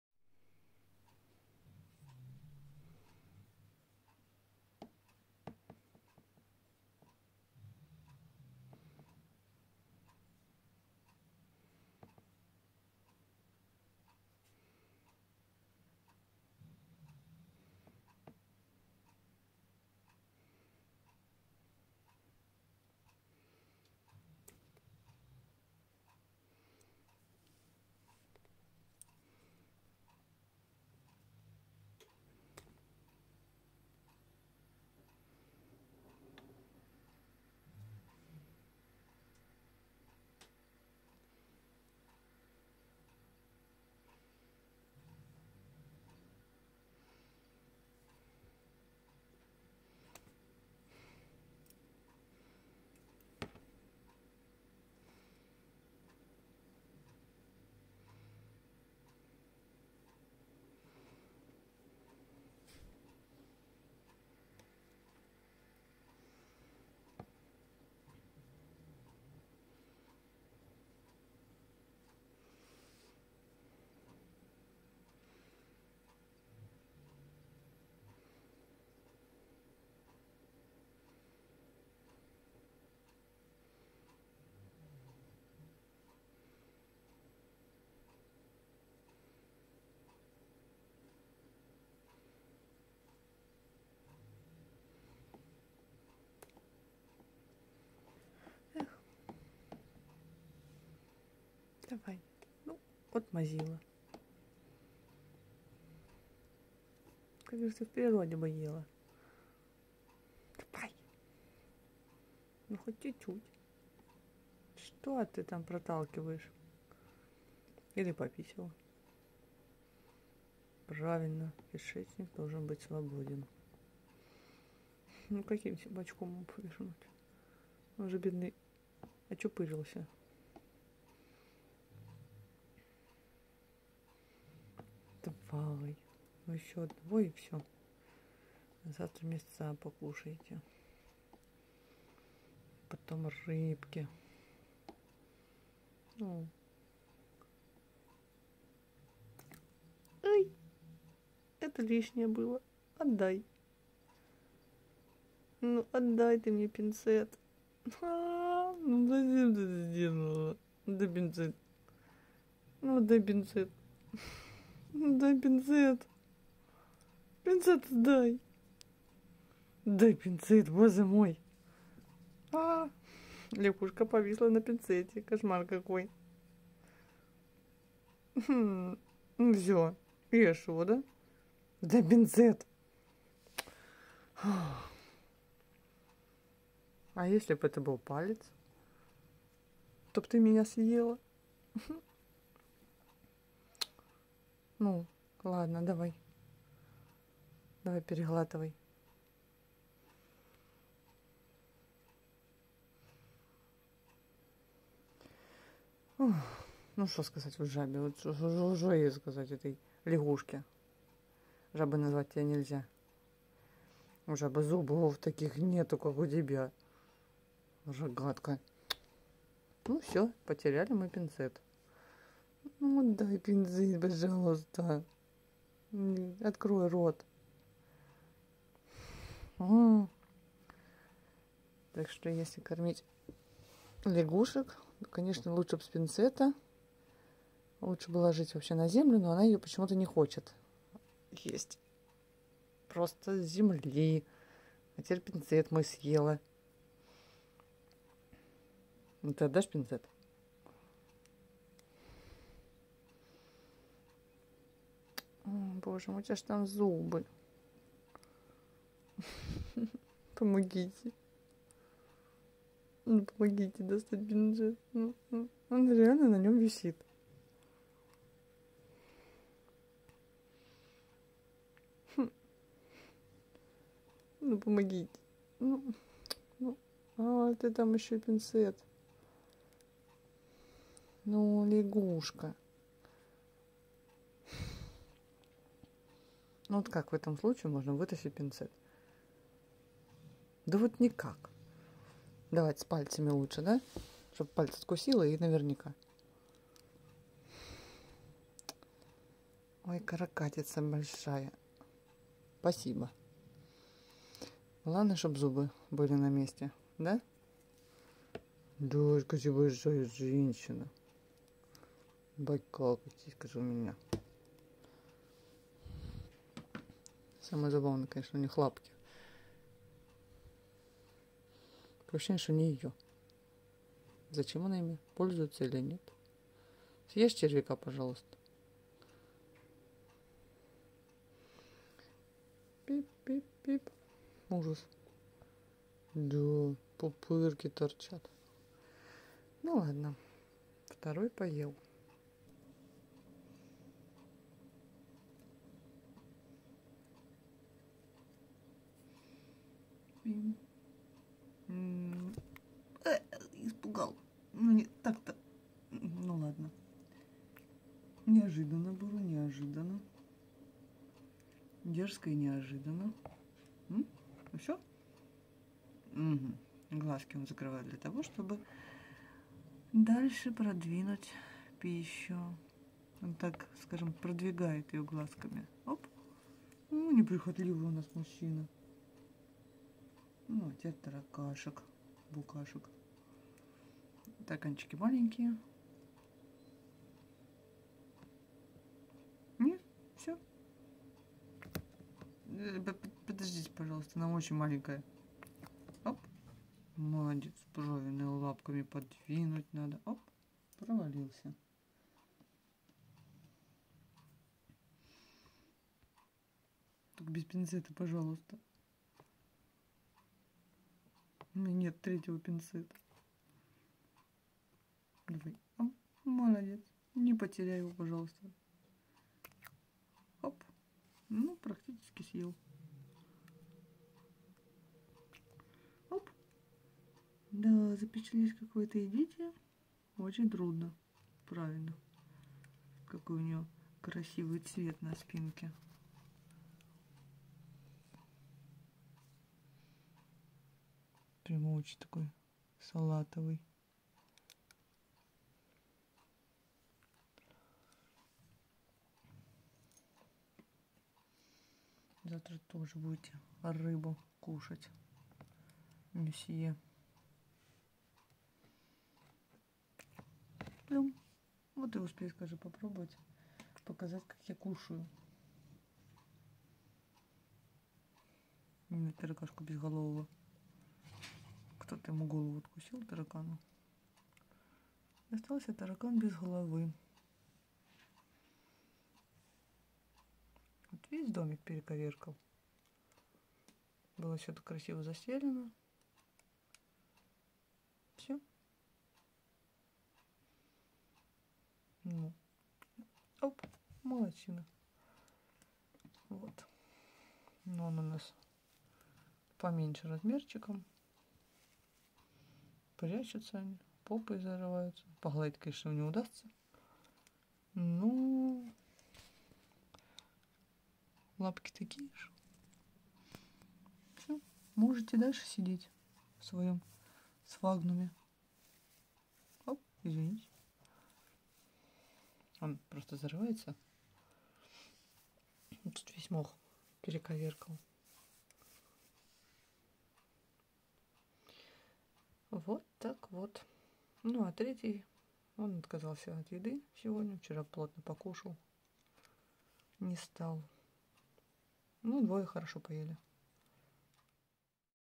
Лягушка Рогатка Кушает
Ииииии всем снова здарова )
 У нас лягушка рогатка, супруга покормила всякими вкусностями , сделала она хорошие наилучшие условия содержания. И  вообще молодец, и лягушка тоже . Смотрим и наслаждаемся =)